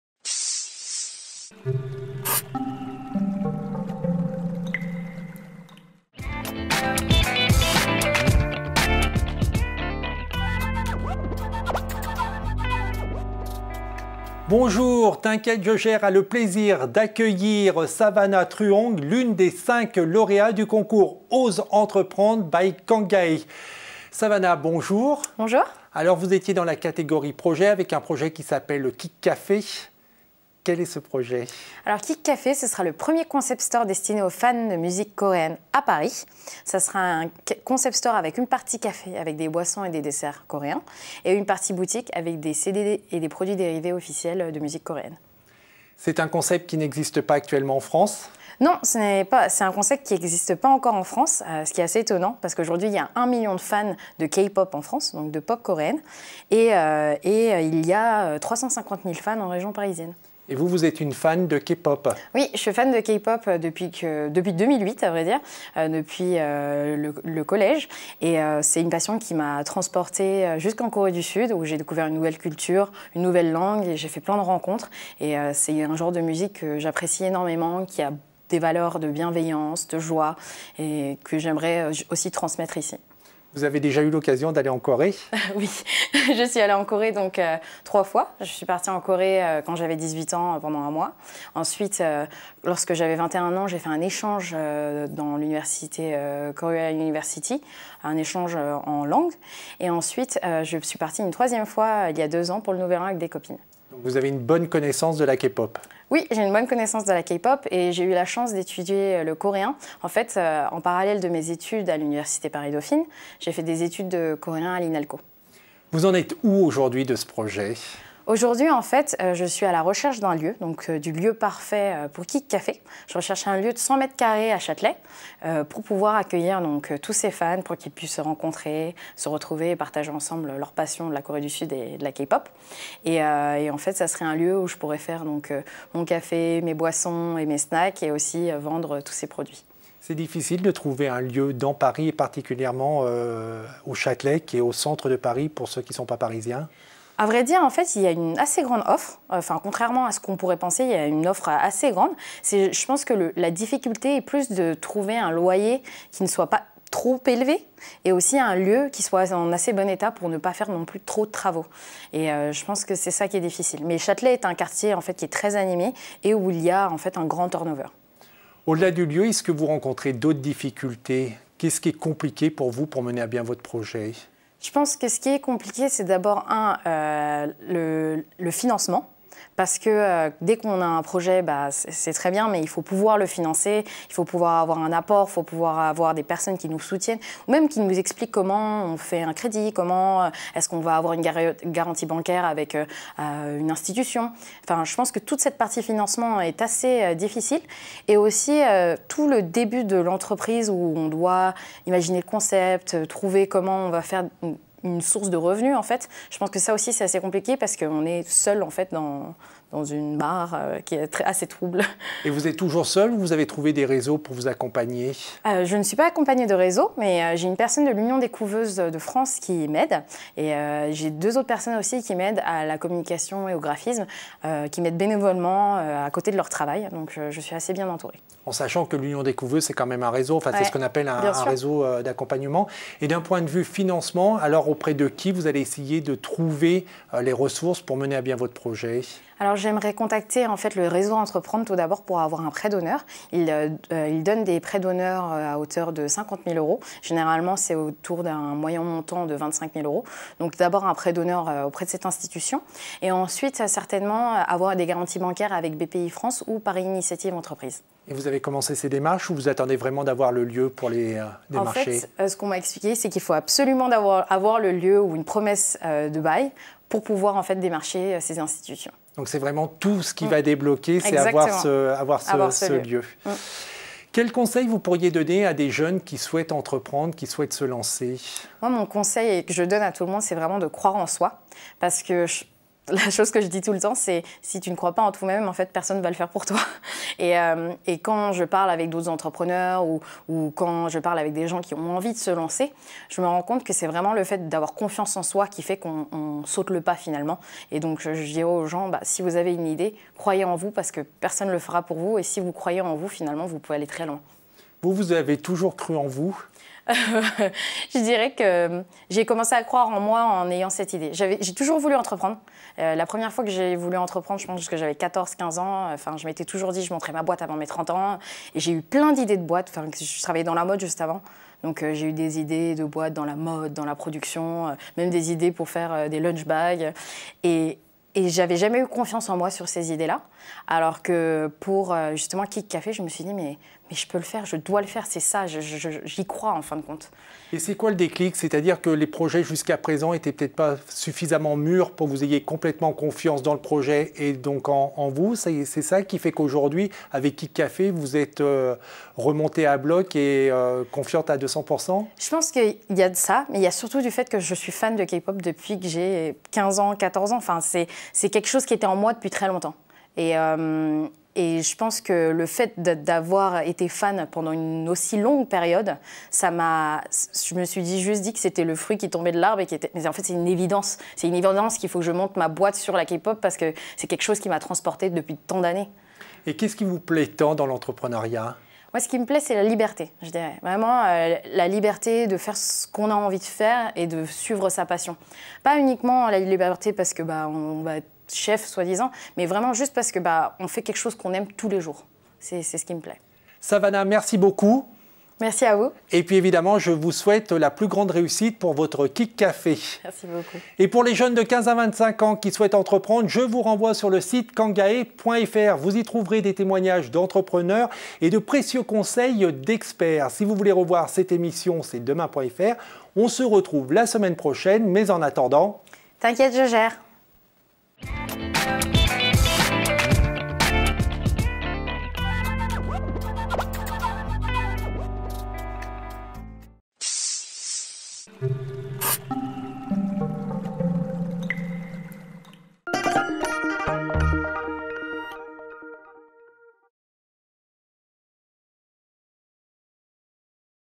"T'inquiète... je gère !" avec Savannah Truong
Savannah Truong a conçu Kick Café, un projet de concept store pour les passionnés de K pop à savoir la musique pop coréenne. Avec ce projet Savannah est lauréate du concours "Ose entreprendre" by Kangae